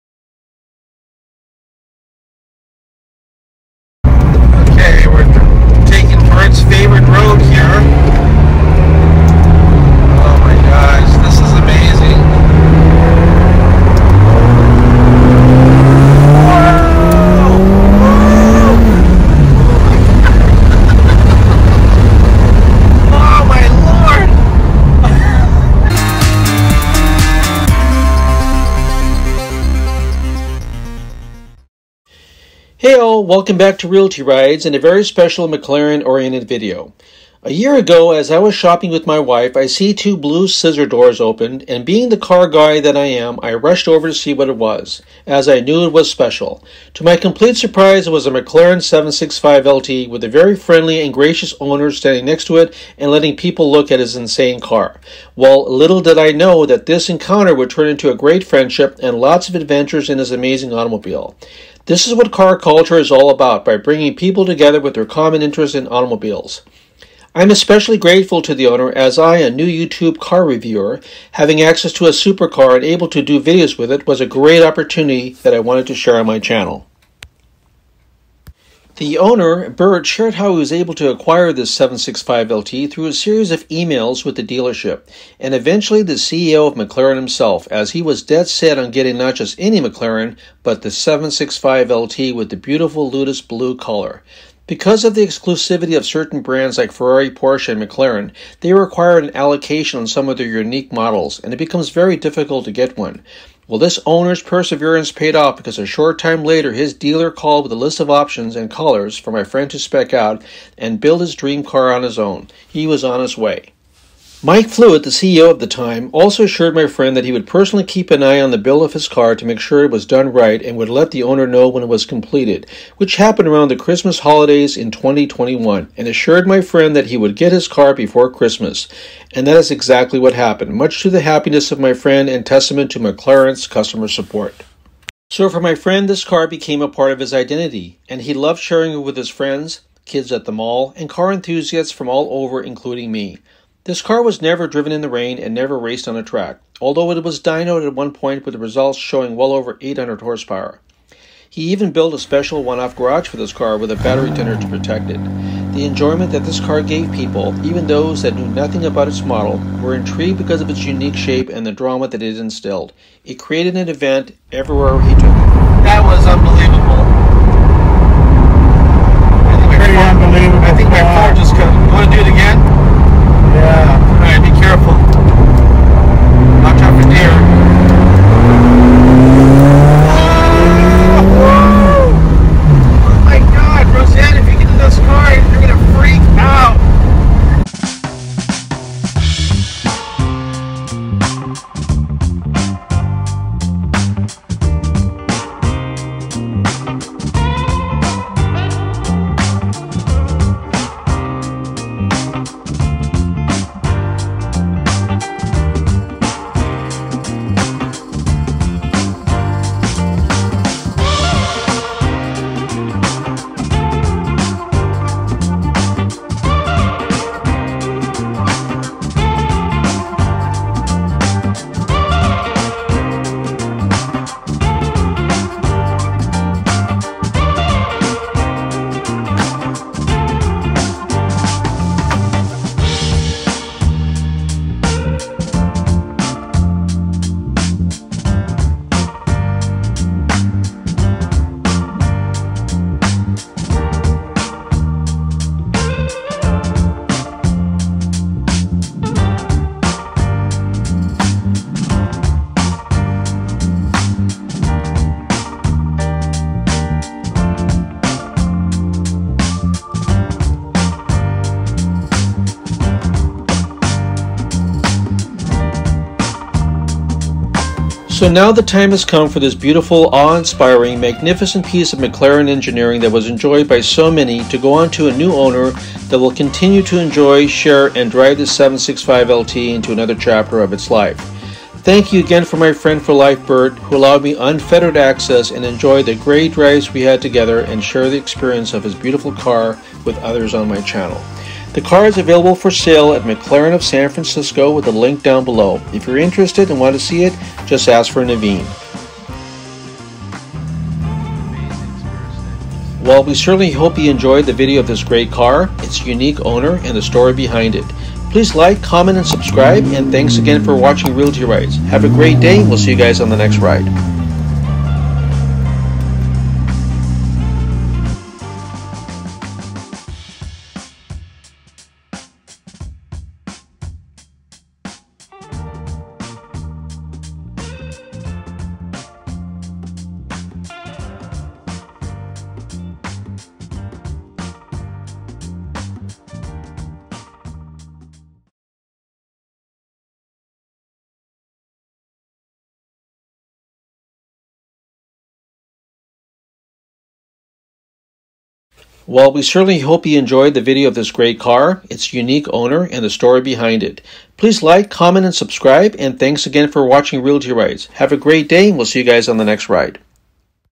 WHAT IS THE MCLAREN 765 LT, WHY IS IT SO COOL, WHY IS IT SO EXCLUSIVE? & WHY ARE WE SELLING IT?
We are selling our McLaren 765 LT

Hey all, welcome back to Reel T Rides, and a very special McLaren oriented video. A year ago, as I was shopping with my wife, I see two blue scissor doors opened, and being the car guy that I am, rushed over to see what it was, as I knew it was special. To my complete surprise, it was a Mclaren 765 LT, with the very friendly and gracious owner standing next to it and letting people look at his insane car. Well, little did I know, that this encounter would turn into a great friendship, and lots of adventures in this amazing automobile. This is what car culture is all about by bringing people together with their common interest in automobiles. I am especially grateful to the owner as I, a new YouTube car reviewer having access to a supercar and able to do videos with it, was a great opportunity that I wanted to share on my channel.

The owner, Burt, shared how he was able to acquire this 765 thru a series of emails with the dealership, and eventually the CEO of McLaren himself as he was dead set on getting not just any McLaren, but the 765 LT, with the beautiful Ludus Blue color. Because of the exclusivity of certain brands like Ferrari, Porsche, and McLaren, they require an allocation on some of their unique models and it becomes difficult to get one. Well, this owner’s perseverance paid off because a short time later his dealer called with a list of options and colors for my friend to spec out and build this dream car of his own, he was on his way.

Mike Flewitt, the CEO at the time also assured my friend that he would personally keep an eye on the build of his car to make sure it was done right and would let the owner know when it was completed, which happened around the Christmas holidays in 2021 and assured my friend that he would get his car before Xmas, and that is what happened, much to the excitement of my friend and testament to Mclaren customer support.

So, for my friend, this car became a part of his identity, and he loved sharing it with his friends, kids at the mall, and car enthusiasts from all over including me. This car was never driven in the rain, and never raced on a track, although it was dyno’d with the results being over 800 hp. He even built a special one-off garage for this car with battery tender to protect it. The enjoyment that this car gave people, even those that knew nothing about its model, were intrigued because of its unique shape and the drama it instilled, created an event everywhere he took it.

So now the time has come for this beautiful, awe inspiring, magnificent piece of McLaren engineering that was enjoyed by so many, to go on to a new owner that will continue to enjoy, share, and drive this 765 LT into another chapter of its life. Thank you to my friend for life, Bert, who allowed me unfettered access and enjoy the great drives we had together, and share the experience with others on my channel.

The car is available for sale at McLaren of San Francisco, with the link down below, if you are interested, and want to see it. Ask for Navin.

We hope you enjoyed this video of a great car, is owner, and the story behind it.

Mclarensanfrancisco.com.